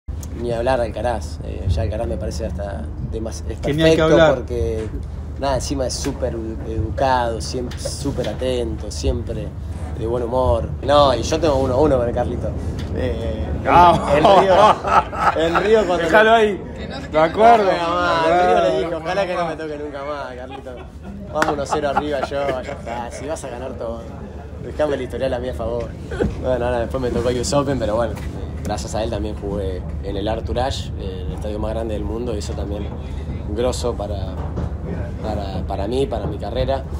Fede Coria habla sobre Carlos Alcaraz
Fede Coria dice que Carlos Alcaraz hace todo bien y que él está 1-1 en el historial personal en una muy amena entrevista con Diego Amuy de BATennis.